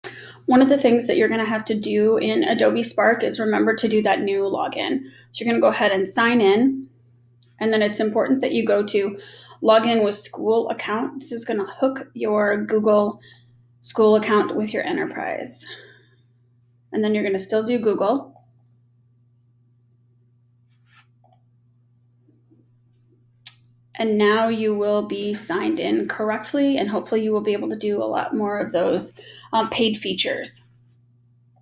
New Sign In - Adobe Spark
How to use the school sign in